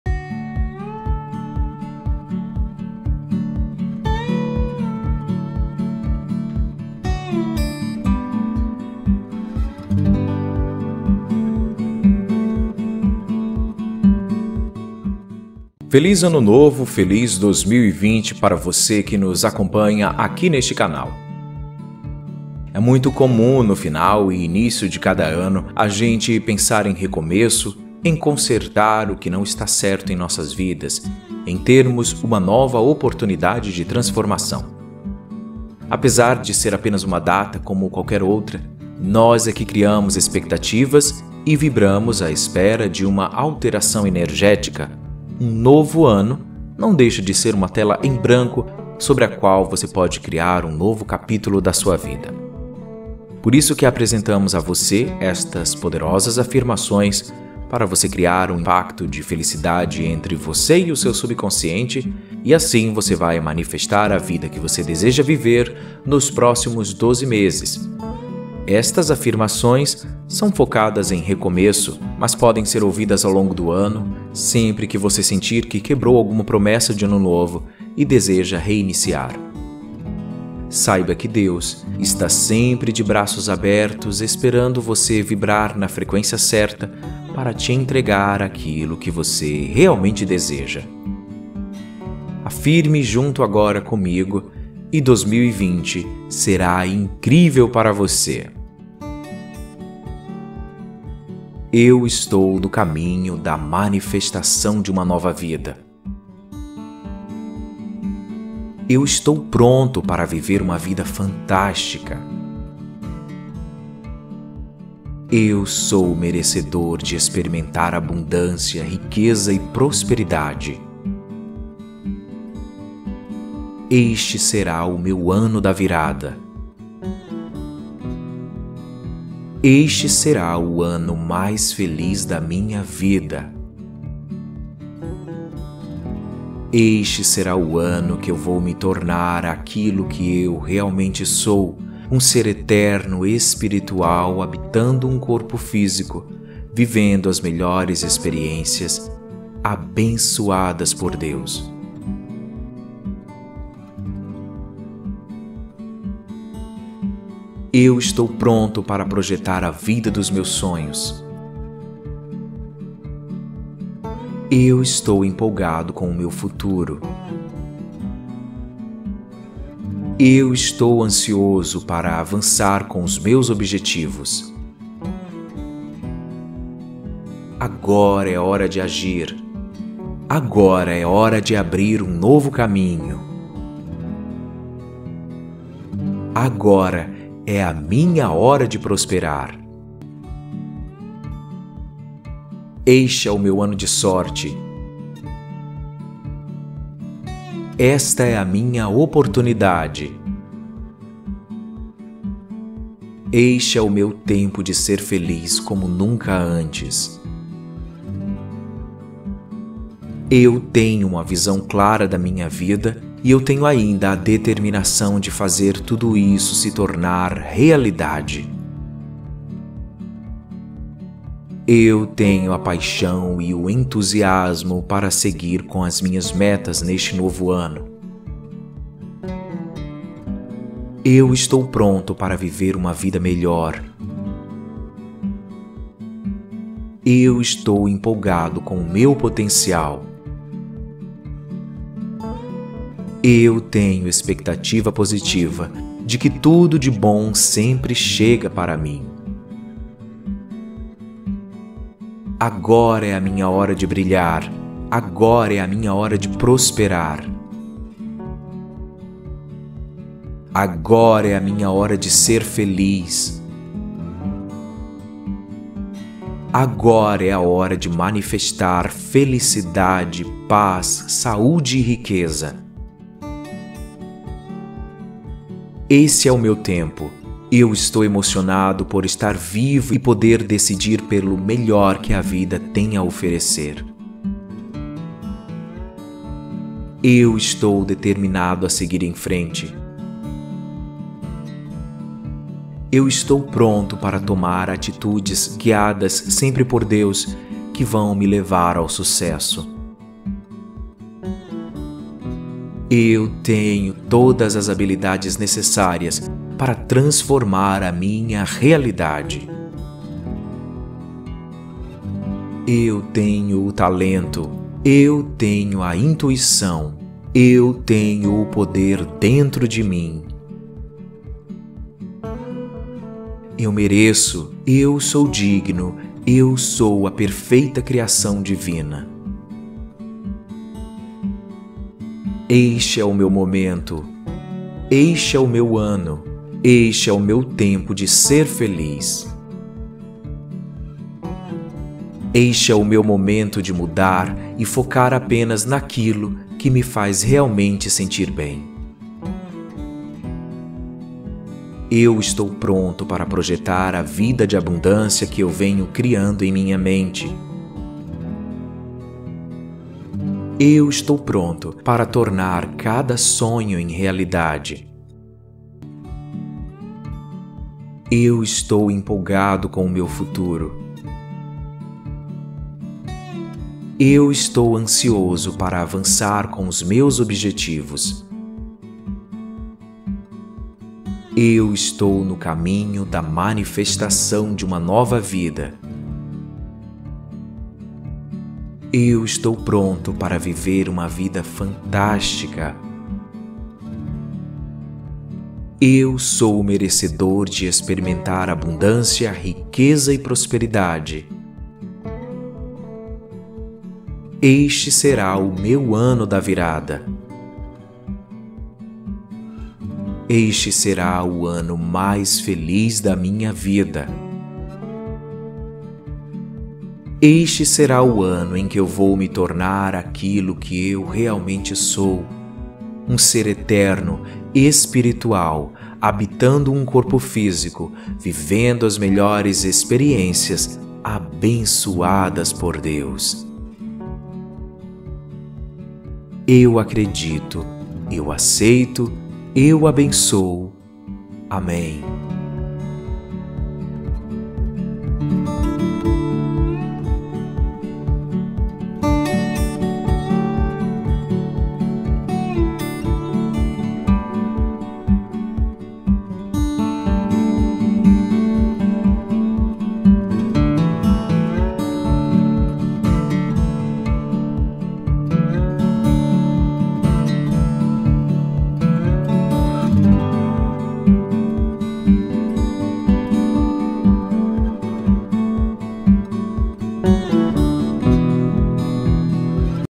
AFIRMAÇÕES POSITIVAS PARA A FELICIDADE EM 2020
É muito comum no final e início de cada novo ano a gente pensar em recomeço, em consertar o que não está certo em nossas vidas, em termos uma nova oportunidade de transformação.

Apesar de ser apenas uma data, como qualquer outra, nós é que criamos expectativas e vibramos à espera de uma alteração energética, um novo ano não deixa de ser uma tela em branco sobre a qual você pode criar um novo capítulo de sua vida.

Por isso que apresentamos a você estas poderosas afirmações para você criar um pacto de felicidade entre você e o seu subconsciente e assim você manifestar a vida que você deseja viver nos próximos 12 meses.